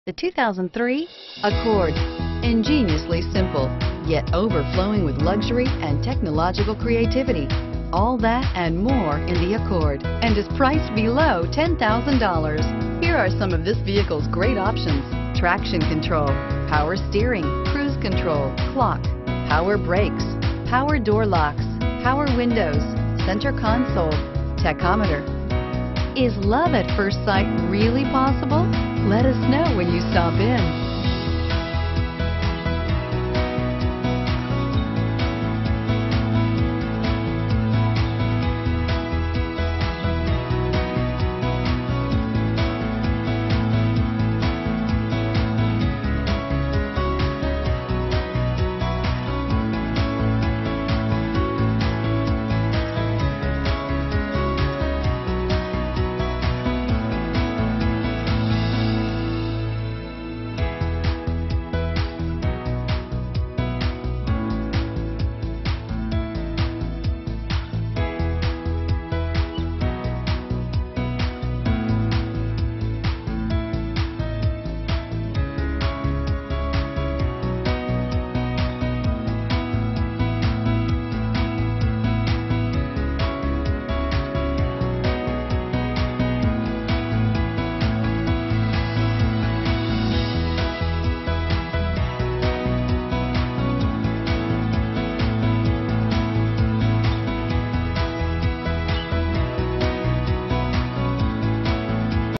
2003 Honda Accord EX V-6 Schaumburg IL R0360A
2003 Honda Accord EX V-6

Schaumburg Toyota
125 W Higgins Rd

Easily practice safe driving with anti-lock brakes and traction control in this 2003 Honda Accord EX V-6.  It has a 3 liter 6 Cylinder engine.  With a 5-star safety rating, this is one of the safest vehicles you can buy.  The exterior is grey.  Dual climate control allows driver and passenger to pick their perfect temperature.  According to a review from New Car Test Drive, Honda might have outdone itself this time.